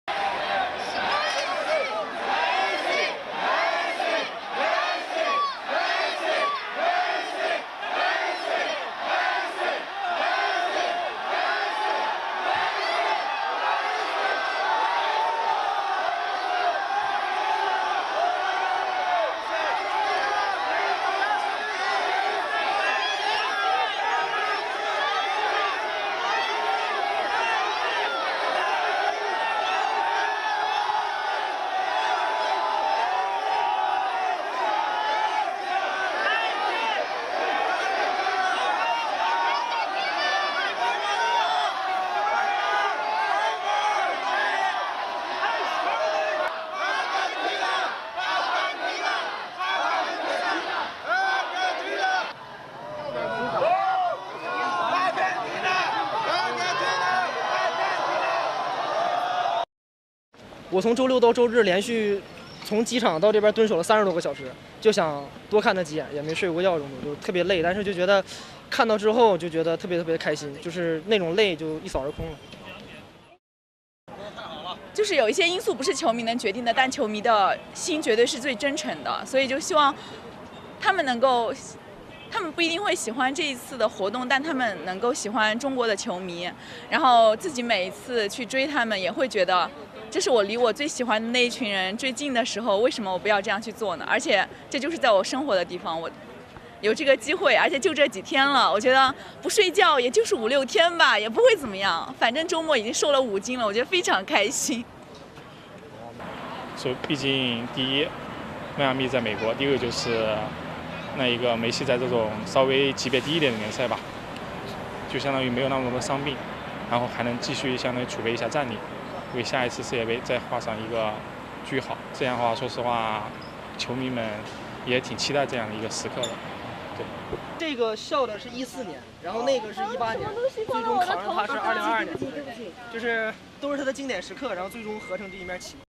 ‘Messi mania’ in China as fans clamor for a glimpse of Argentina star ｜Selección Mayor
Hordes fans from all over China gathered at Beijing Four Seasons Hotel in East Beijing on Monday evening, hoping to get a glimpse of their hero – Lionel Messi and Team Argentina which is playing a friendly game with Team Australia in Beijing Workers’ Stadium on Thursday, 15th June.
Messi and most of the Doha World Cup winners arrived in Chinese capital on Saturday, tens of thousands of local fans started chasing the international soccer star with emotion and enthusiasm from the Beijing capital airport to the Four Seasons hotel that the Argentine National team is staying during their China trip.
Argentine squad had a training on Monday afternoon at Beijing Aoti Sports Center, when the team bus came back to the hotel, fans started waving to the bus and chanting “ Messi”, “ Argentina”.
The Argentina star, sitting at the back in the bus, looked at the fans through the window glass and waved to the exciting fans.
Hundreds of fans raised their phones as high as possible to take pictures or videos of the bus, hoping to get their idol in the frame so they can show off in their social networks.